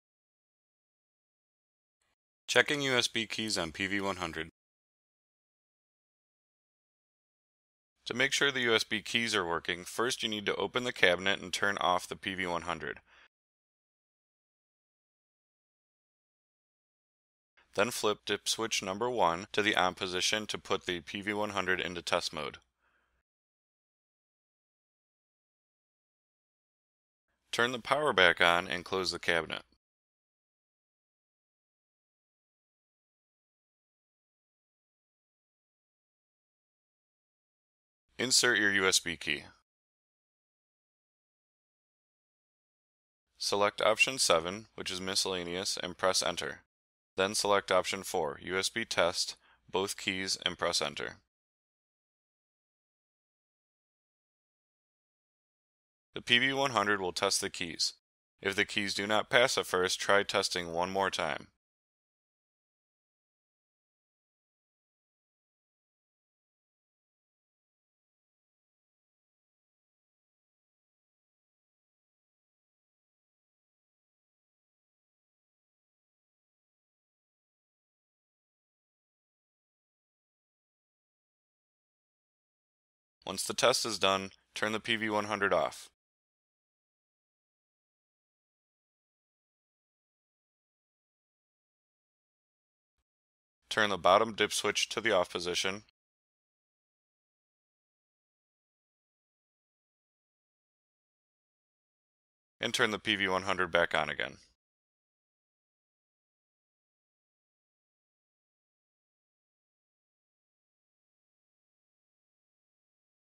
Verifying USB Keys on the Petro Vend PV100
How to check or verify the USB keys are operating correctly on PV100